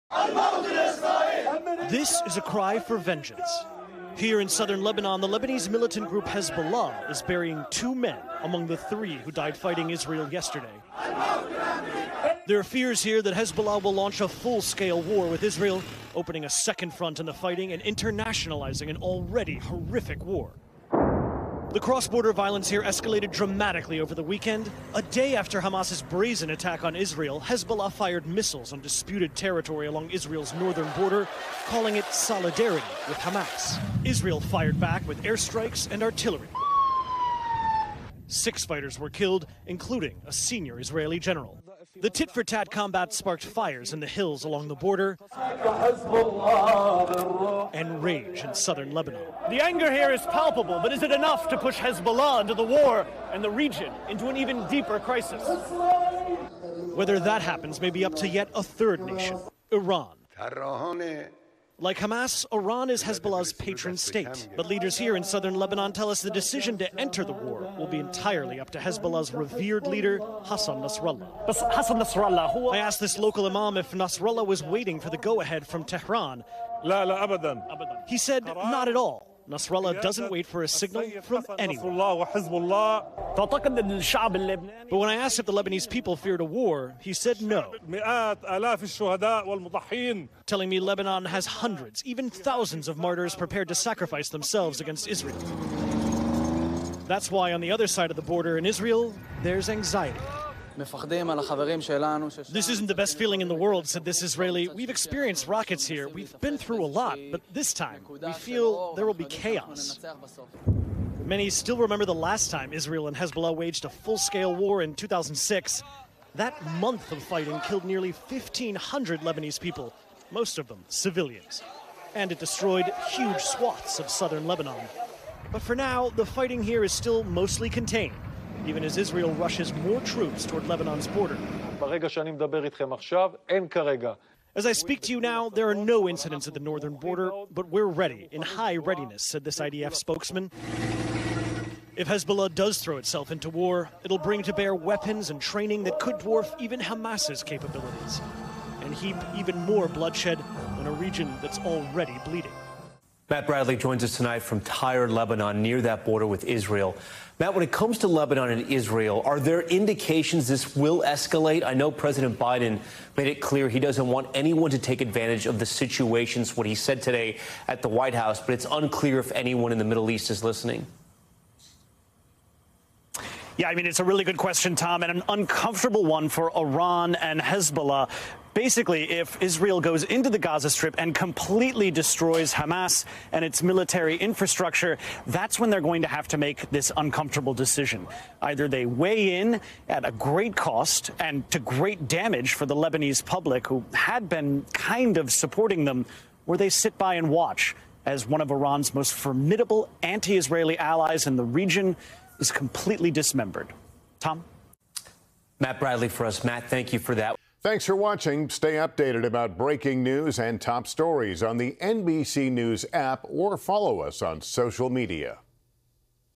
Fears of escalation arise as Israel and Hezbollah exchange fire
Fears are growing that Hezbollah will initiate a war with Israel after two members of the militant group were buried in southern Lebanon after exchanging fire with Israel.